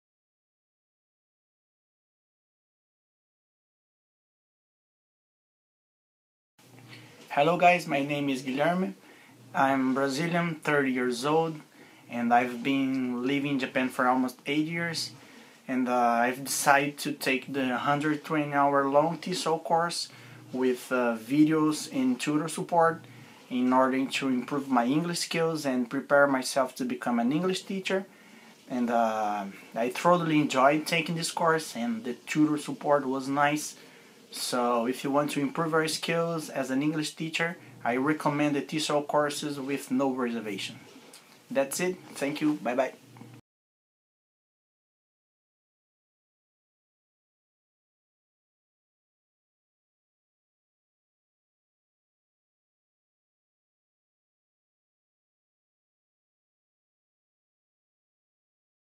TESOL TEFL Reviews - Video Testimonial – Guilherme
In this TESOL review video Guilherme speaks about his experience with ITTT. He recently took our 120-hour TESOL course with videos and tutor support to become an English teacher in Japan. He enjoyed the course and also thought that the tutor support was very helpful for completing the course.